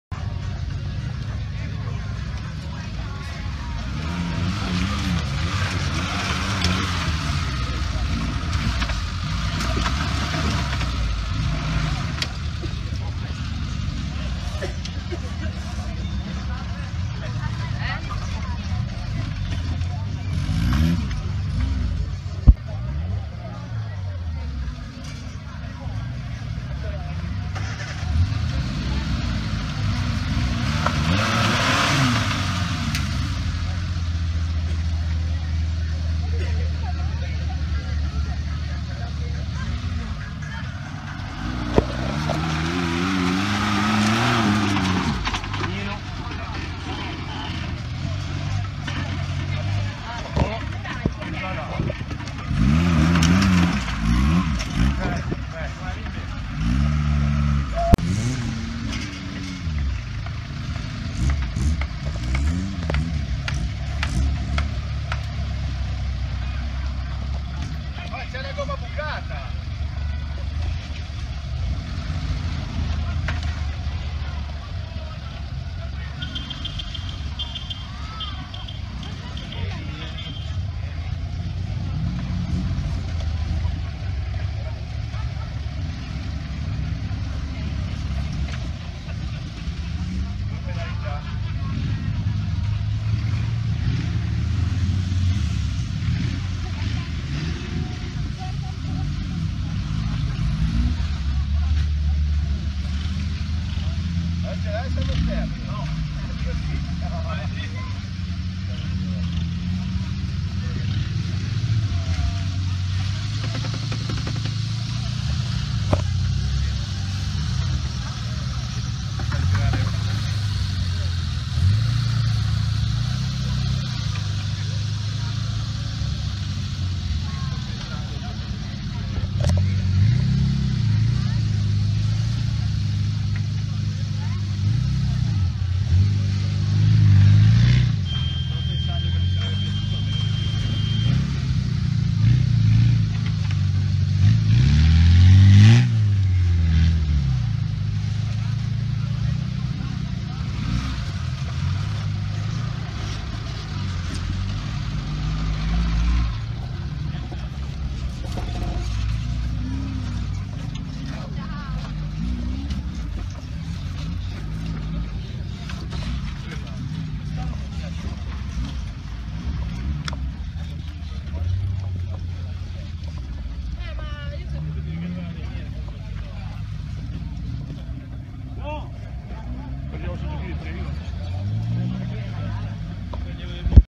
Raduno 4x4 bosio 2015
Sistemato dopo il cappottamento e aggiornato con alcune modifiche il Suzu torna in pista al raduno di bosio.